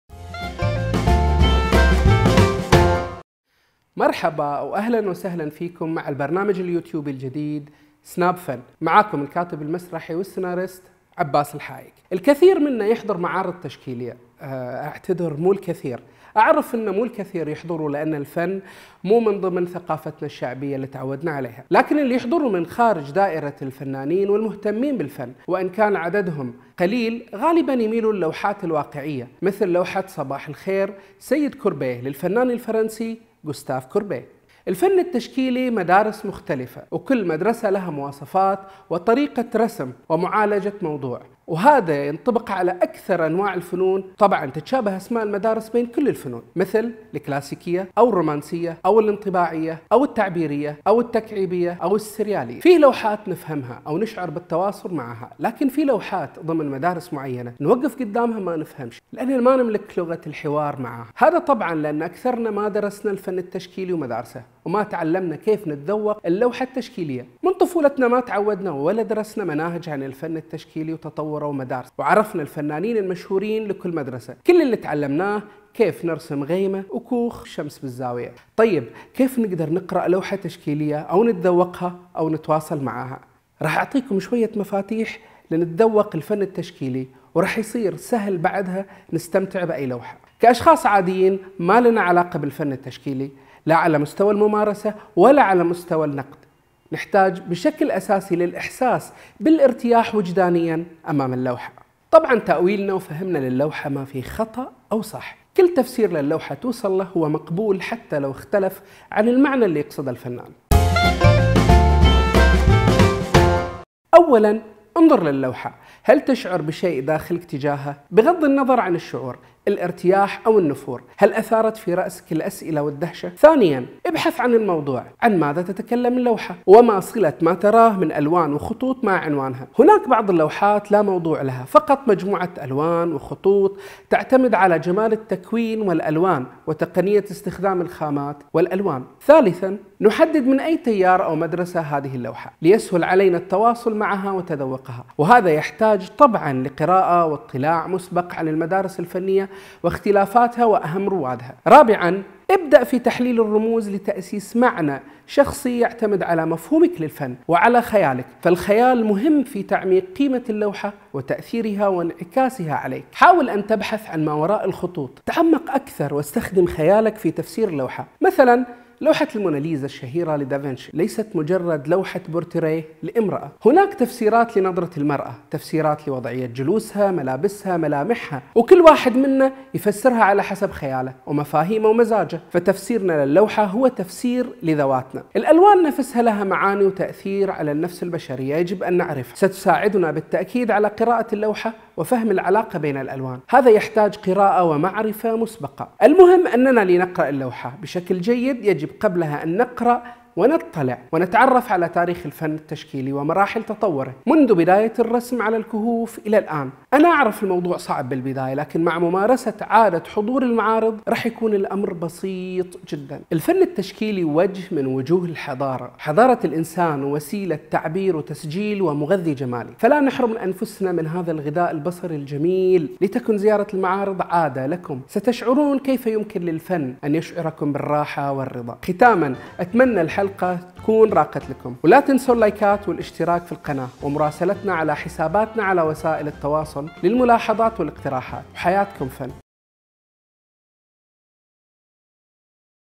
كيف تقرأ لوحة تشكيلية ؟
برنامج ثقافي، يبسط المفاهيم الثقافية والفنية.
إعداد وتقديم:
عباس الحايك @abbas_hayek
جرافيكس:
عبد الخالق آل سلام
مونتاج إخراج:
مهدي آل زيد
*ملاحظة: اللوحة 2 للفنان التشكيلي السعودي زمان جاسم